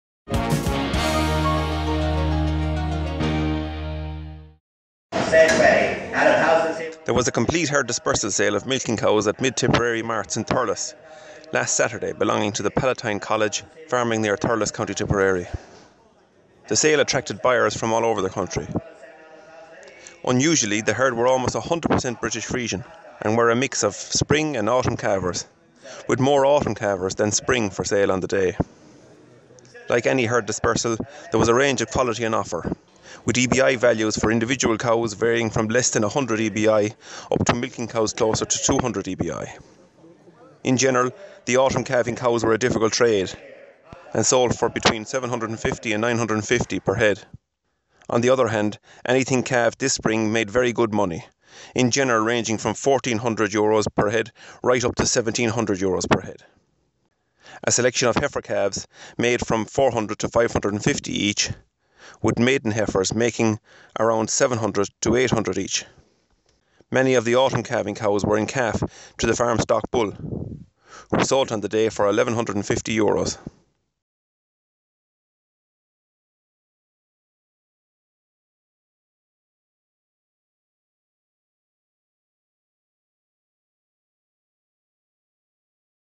Dairy cow auction at Mid-Tipperary Marts
Jack Kennedy reports on a complete herd dispersal sale of milking cows at Mid-Tipperary Marts in Thurles last Saturday on behalf of the Pallottine Fathers, Pallottine College, Thurles, Co Tipperary.